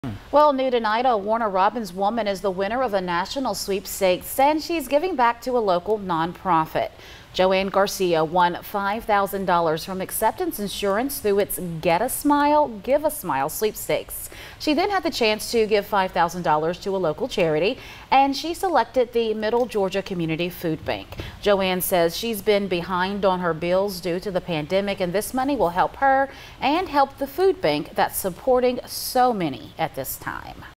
Warner Robins woman wins $10K, gives portion to local charity
WARNER ROBINS, Georgia (41NBC/WMGT) - A Warner Robins woman wins big in a national sweepstakes, and some of her money is going to a local charitable organization.